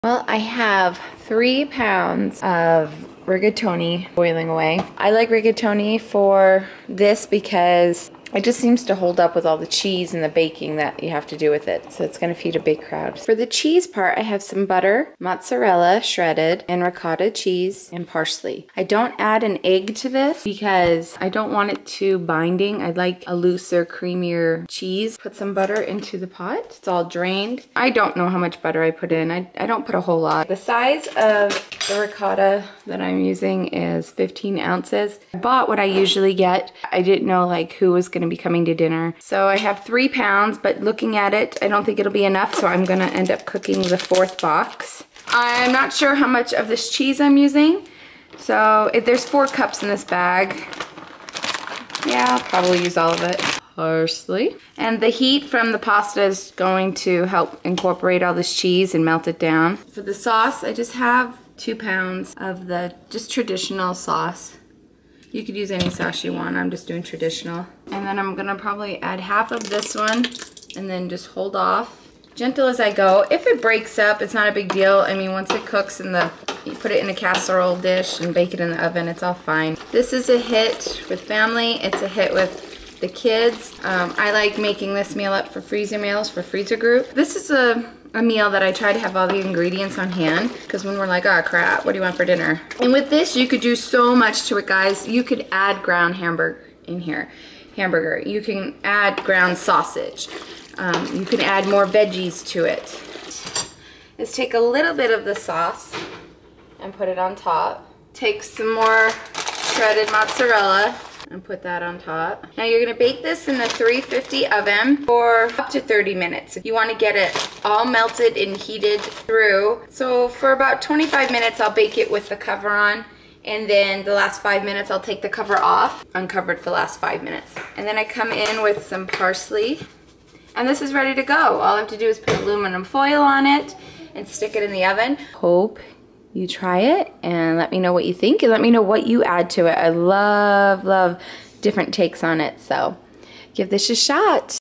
Lazy Man Lasagna-Easiest Lasagna EVER!
Lazy man Lasagna Is the easiest lasagna to put together.  It feeds a crowd.

🍽️Snag The Recipe:
1 lb. rigatoni
16 oz ricotta cheese
2 cups shredded mozzarella cheese
parsley
jar of sauce
350-degree oven for up to 30 minutes. Last 5 minutes, remove the cover and bake.

▶Videos Mentioned / Related